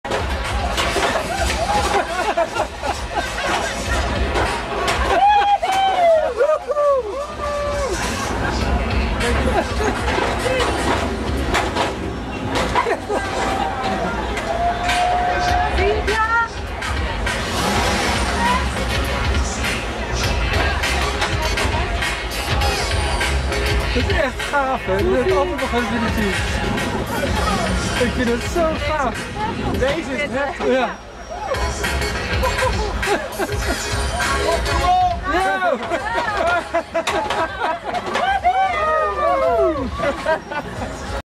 let's rock 'n roll!
Driver is shouting out to the public: 'Let's rock 'n roll' and lets his car jump through the street